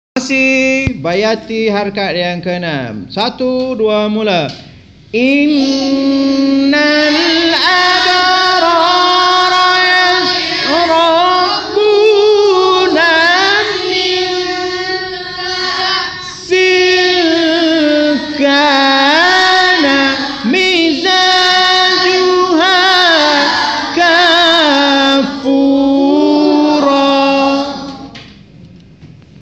formasi tarannum BAYATI HARAKAT 06 al insan
kelas ini setiap ahad di batu muda tanbahan jam 2.30pm,
ianya menerapkan pergerakan formasi dalam alunan...aaammat menarik...selamat mencuba...